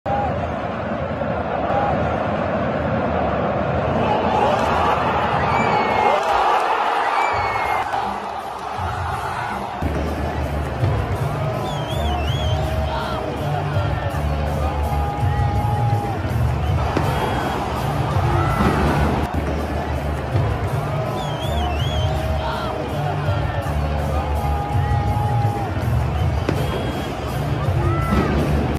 OhWhatAGoal! from Dimitri Payet in Olympique de Marsella vs PAOK
Oh what a goal! from Dimitri Payet vs PAOK in uefa conference league quarterfinals